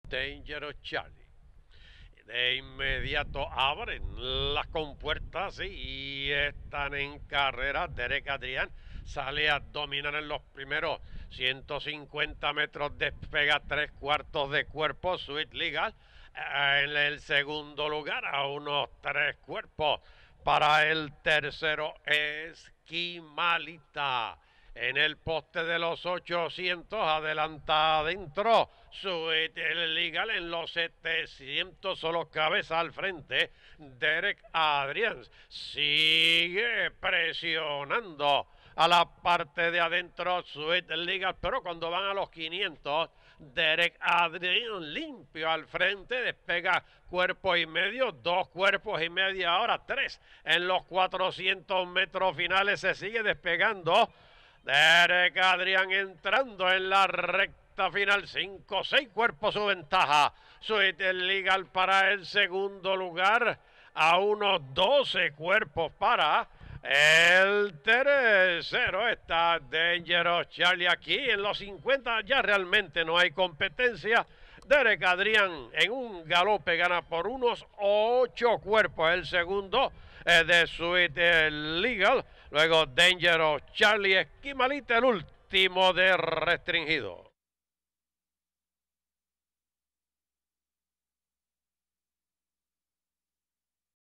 DEREK ADRIAN - Primera Carrera - 28 Abril 2017
El ganador clásico importado Derek Adrián, montado por Erik Ramírez con 120 libras, se impone ante cuatro rivales por reclamo de $20 mil durante la primera carrera del viernes 28 de abril de 2017 en el hipódromo Camarero, de Canóvanas, Puerto Rico, para el Aquino Stable y el entrenador Cristian Díaz. Cronometró 1:13.57 para 1,200 metros luego de fraccionales de :24.16 y :47.66.
ORDEN DE LLEGADA:
1 - Derek Adrián - Erik Ramírez 120 lbs.
2 - Sweet Legal - David Rosario 118 lbs.
3 - Dangerous Charlie - Sashá Ortiz 116 lbs.
4 - Eskimalita - Juan Antonio Santiago 117 lbs.
5 - Restringido - Ismael Pétrez 116 lbs.